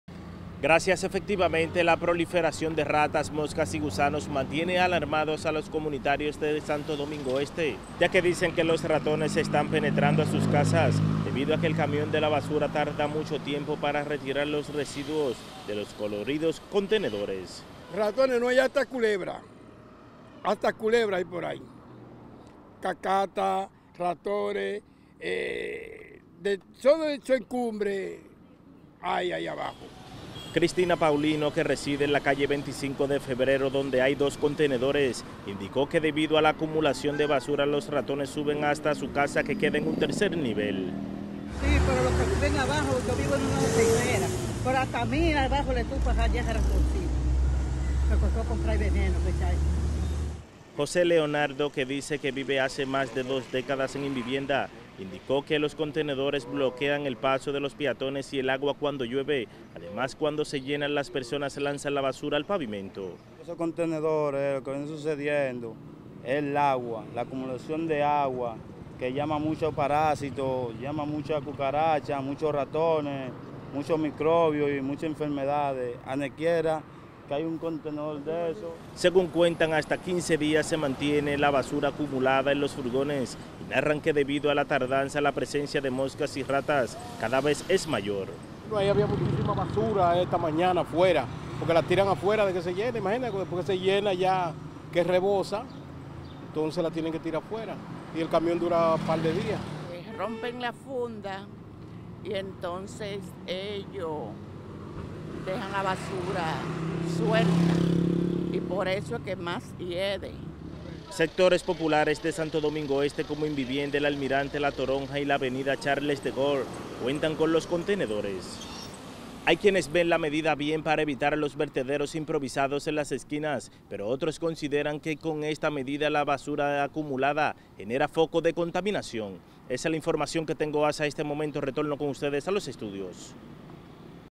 Contenedores instalados por ayuntamiento de convierten en focos de plagas
La instalación de grandes y coloridos contenedores para la basura en Santo Domingo Este, como una solución urgente a los problemas de desechos en ese municipio, se han convertido, según sus residentes, en un foco de plagas y enfermedades, ya que los cúmulos de basura pestilente permanecen dentro de los furgones por varios días sin que los camiones recolectores los vacíen.